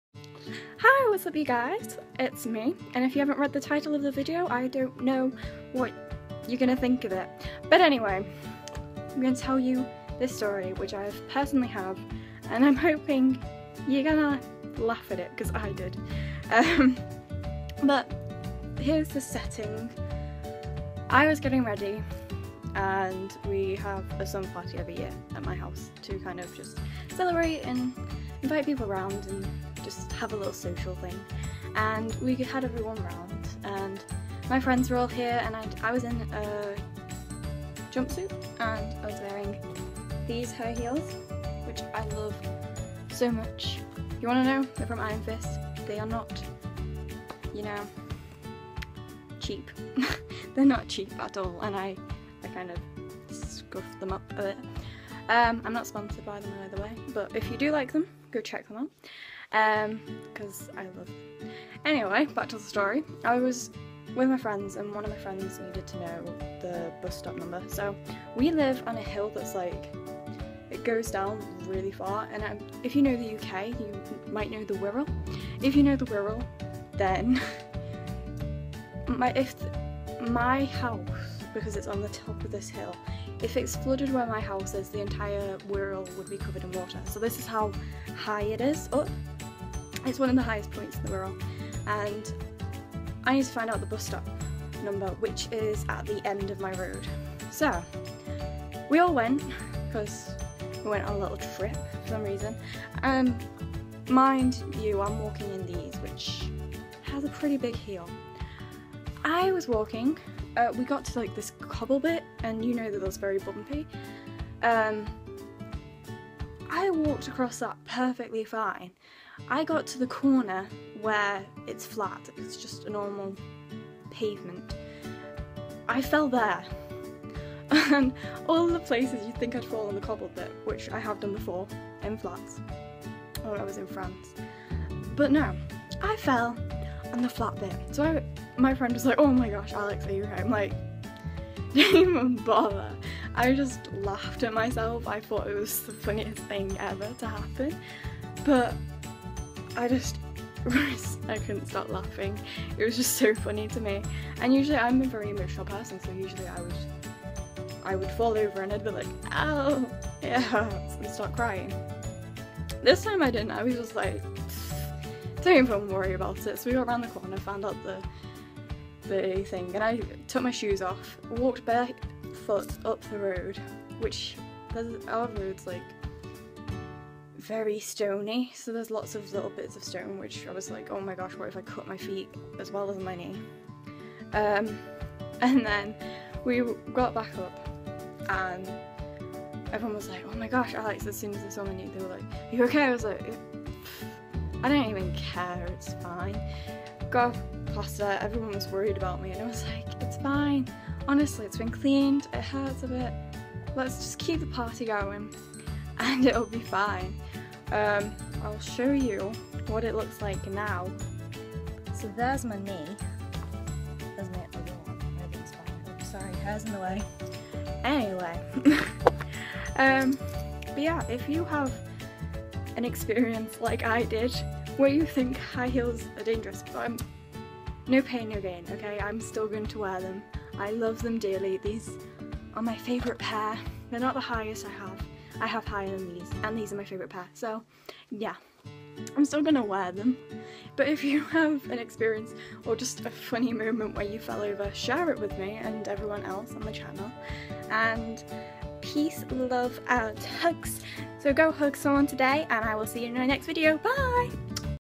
Highheels are dangerous
This is just a little story about me that I thought was funny 😄 I hope you enjoy it